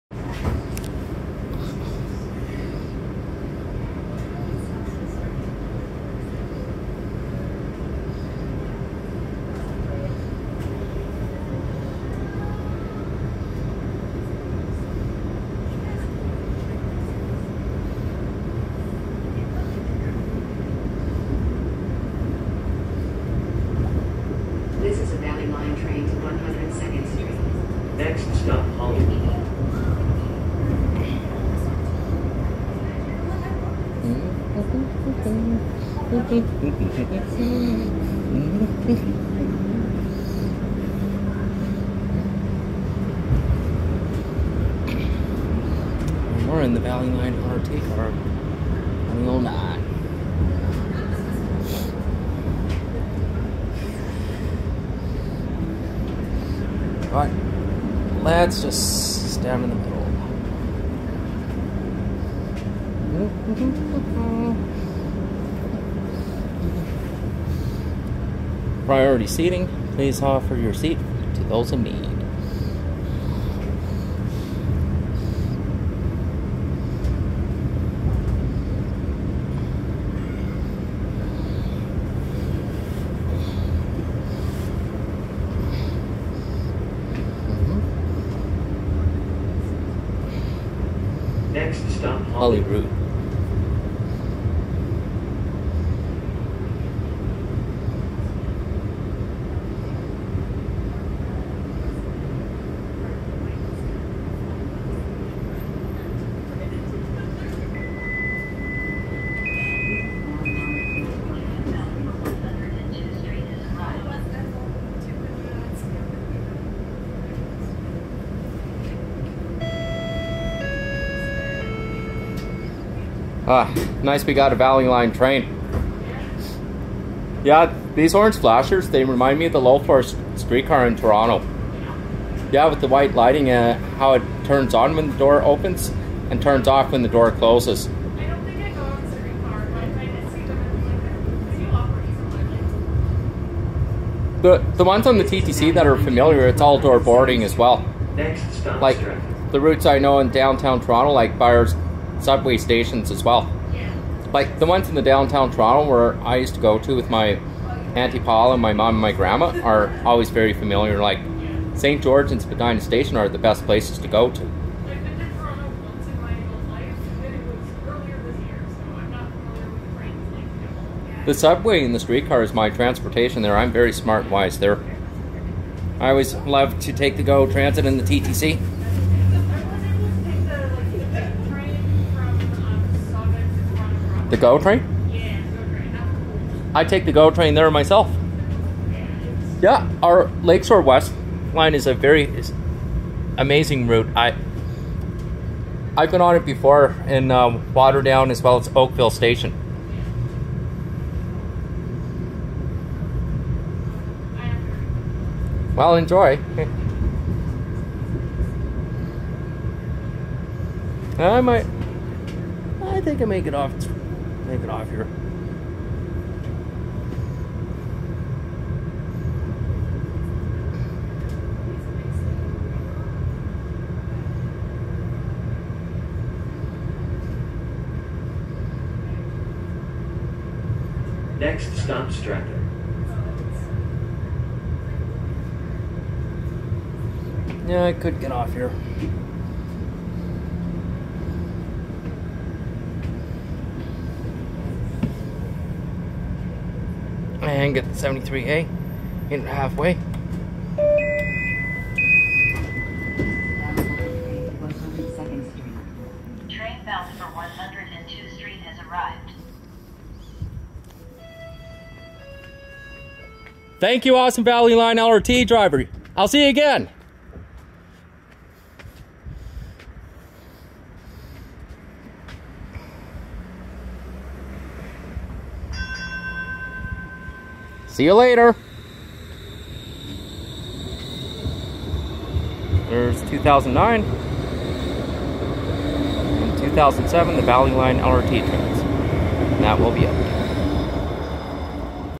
ETS LRT Bombardier Cars #2009 & #2007 (Valley Line) Northbound
From Bonnie Doon Station Outside Bonnie Doon Mall To Strathearn Station, in Edmonton Alberta.